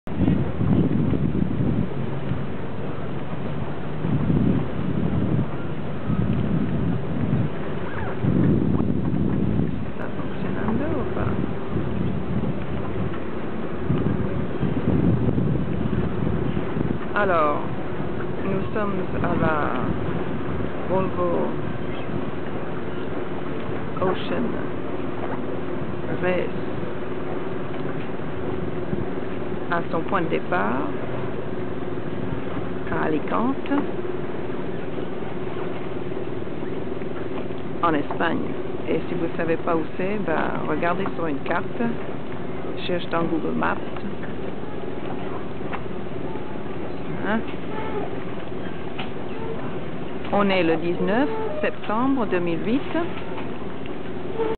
Volvo Ocean Race Village, Alicante, Spain
270º view of the Volvo Ocean Race Village in Alicante. Alicante is host port for the departure of the round the world sailing race , Volvo Ocean Race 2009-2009 edition.